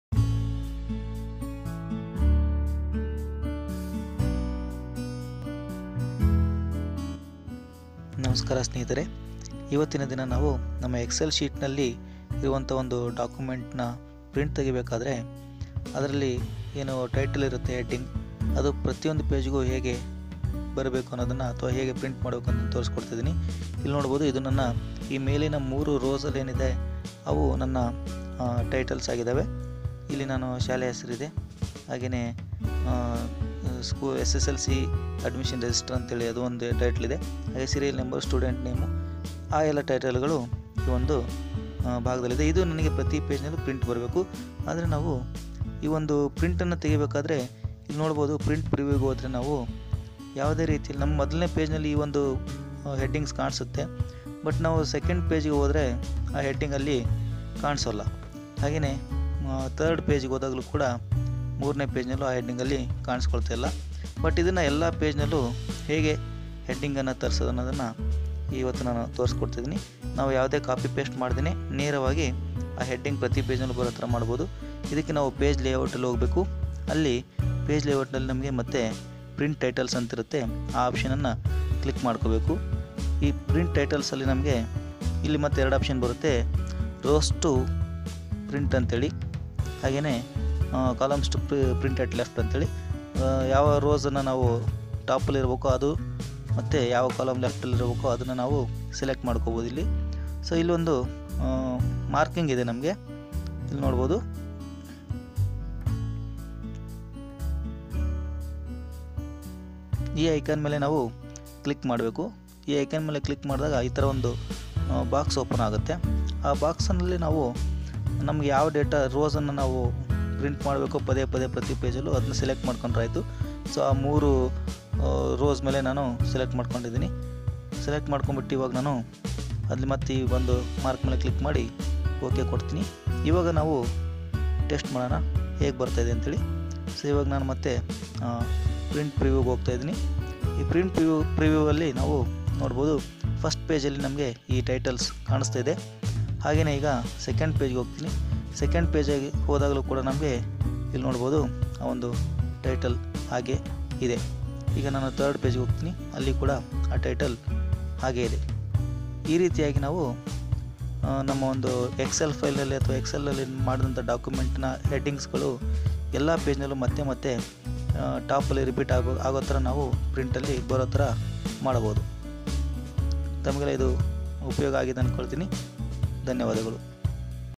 How Print Titles on Top of Each Page during printing a MS Excel document
No need to copy paste titles at the top of each individual page. Just use this trick and print titles on top of each page while printing an excel document.